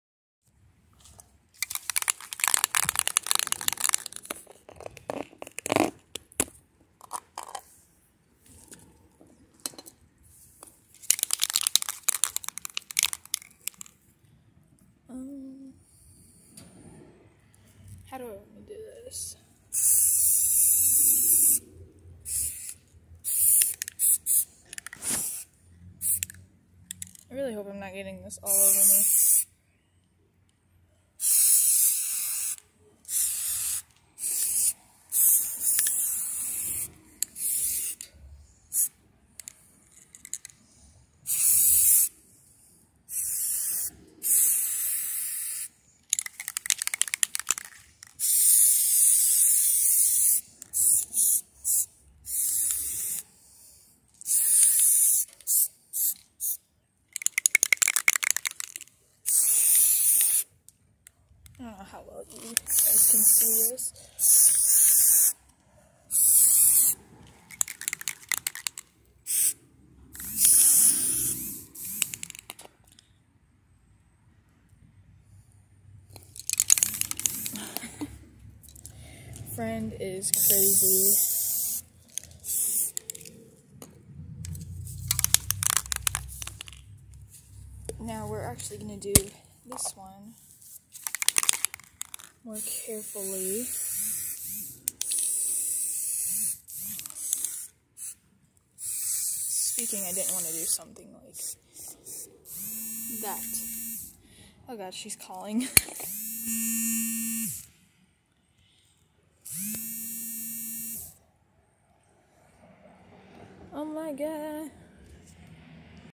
Spray Paint - Daily Vlogs
I was spray painting this rack/coat thing I got for my room, I ended up doing the whole thing purple. I decided to record then someone called me. I can't remember who it was.

Thumbnails will be worked on, I am working on editing, hopefully.